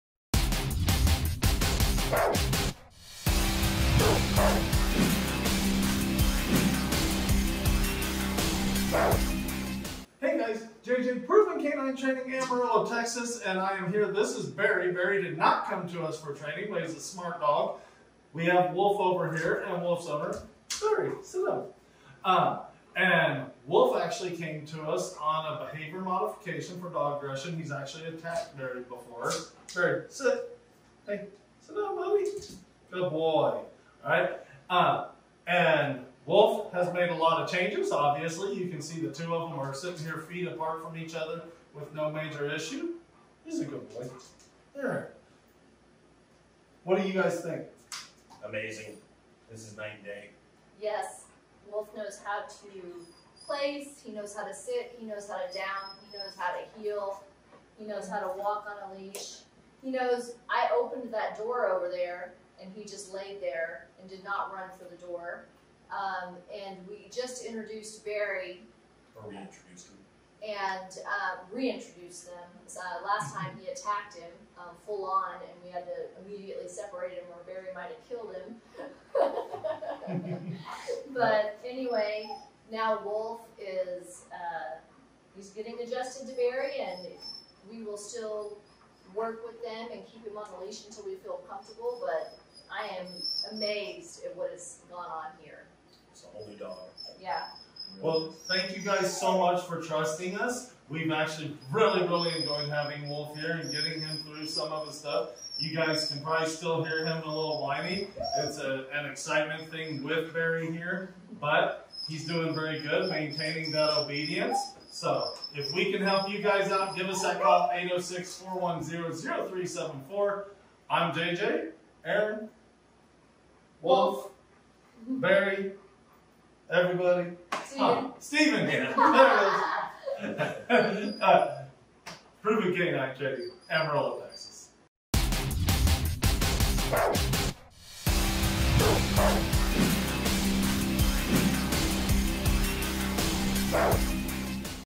Wolf, Behavior Modification, Testimonial upon completion of training.
Wolf went home on Monday!  Wolf was with us for a behavior modification, due to a couple of attacks against Barry.  Listen to what Wolf's owner has to say!